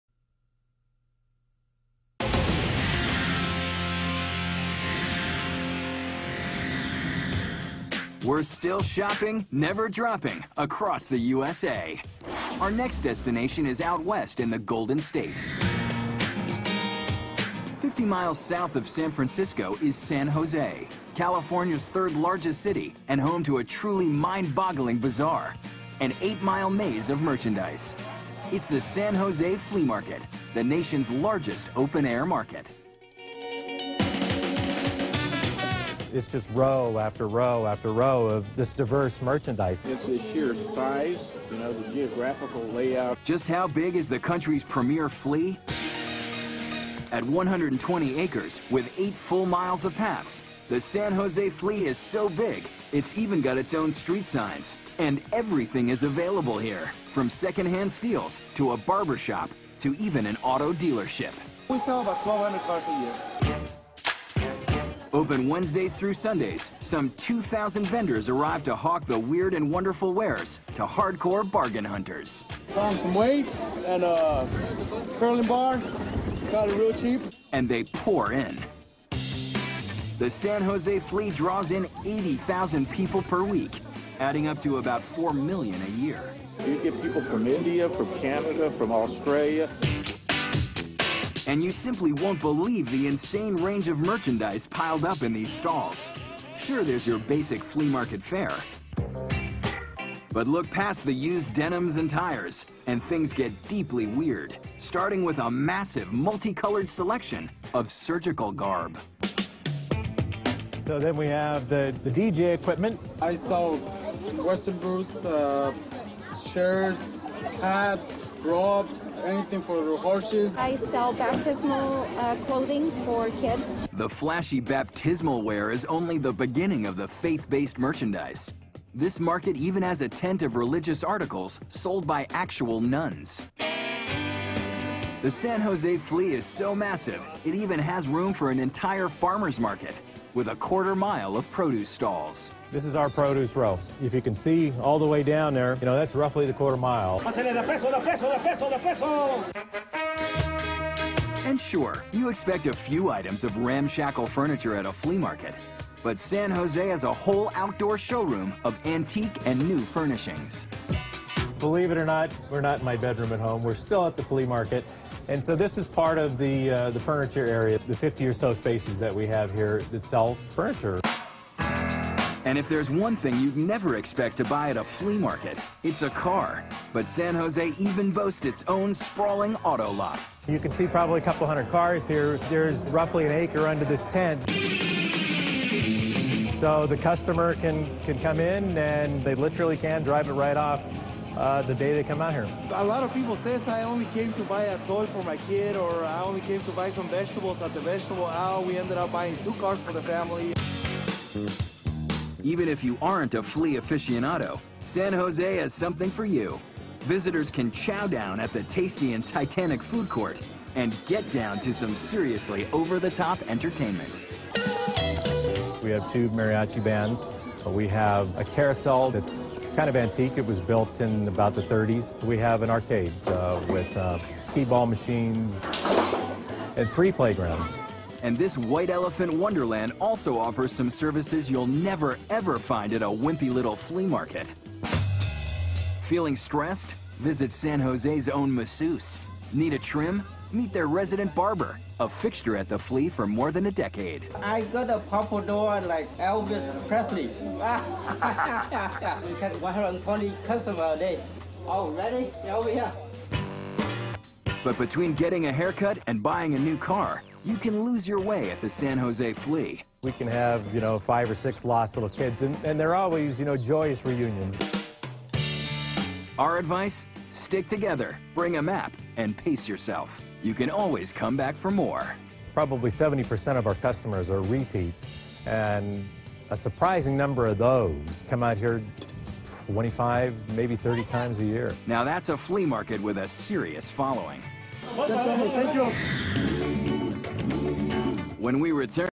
Travel Channel Explores the San Jose Flea Market
Travel Channel highlights the treasures and possibilities at the San Jose Flea Market.